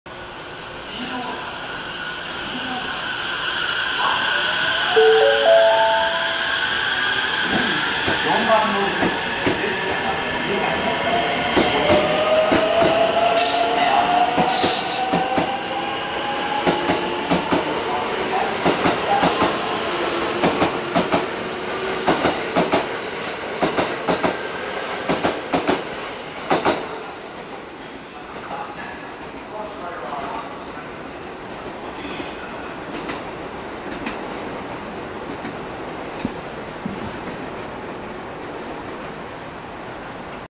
★20240406 225×225系 発車 ＠新大阪
撮影路線：JR西日本 東海道本線(JR京都線)
撮影駅：新大阪
行先：A新快速 野洲
車両：225×225系
両数：4+8両＝12両
MH：なし
備考欄：